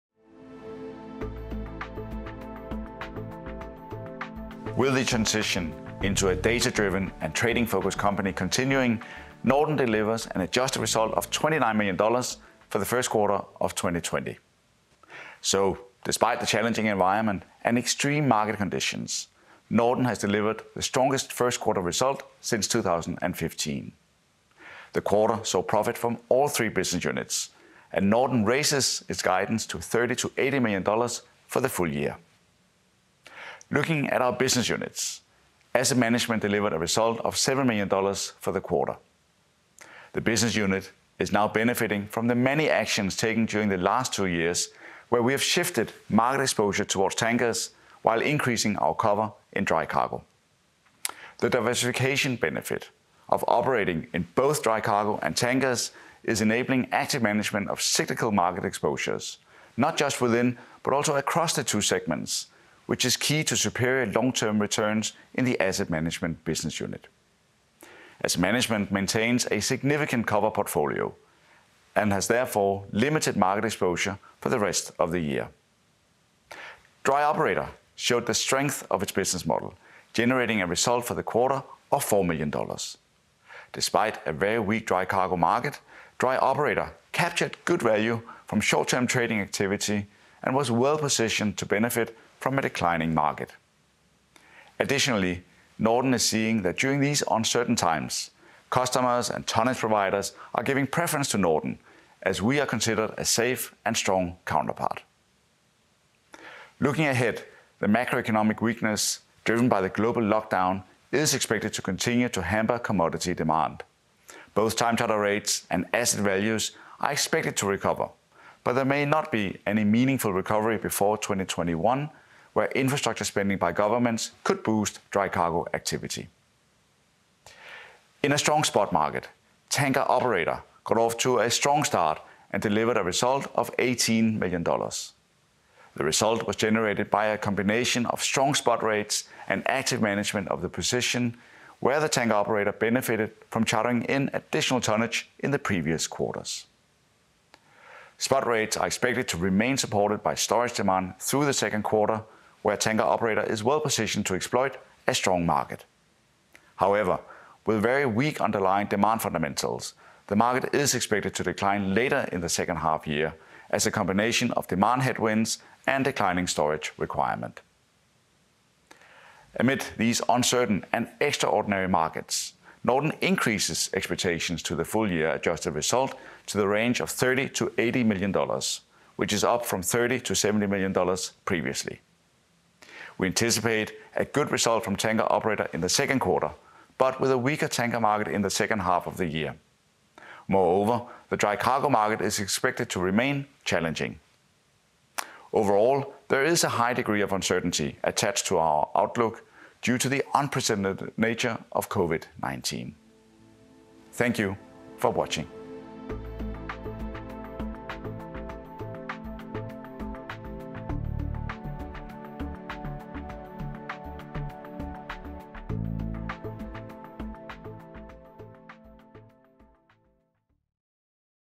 NORDEN update 1. kvartal 2020
Adm. direktør Jan Rindbo præsenterer NORDENs 1. kvartal 2020. På engelsk med danske undertekster.